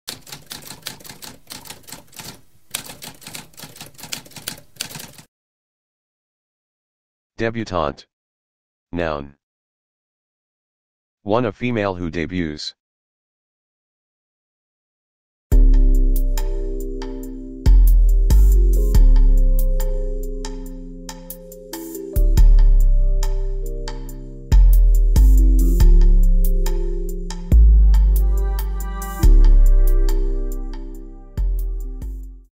What does débutante mean?
A spoken definition of débutante.

Intro Sound:
Typewriter - Tamskp